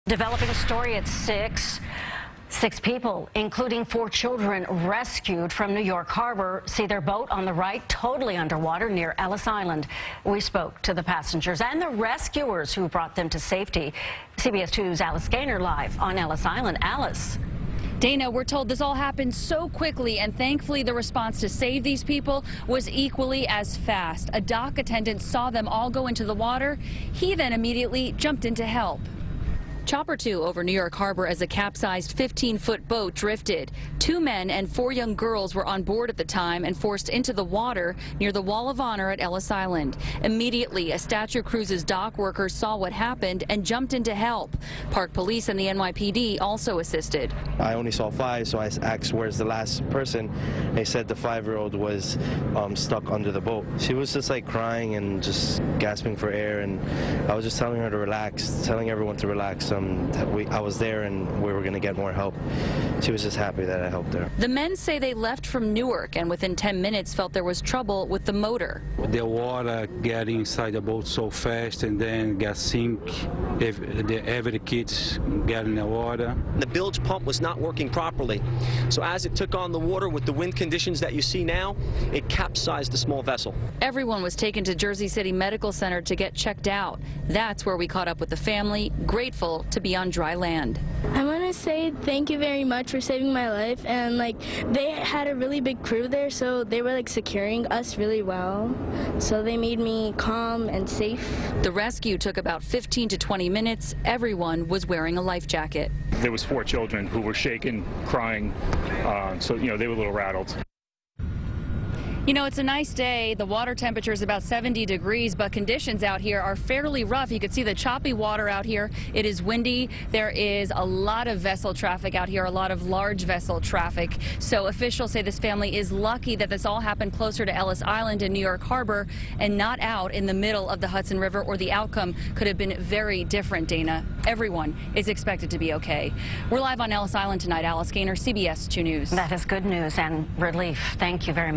Boaters Rescued From NY Harbor
CBS2’s Alice Gainer reports.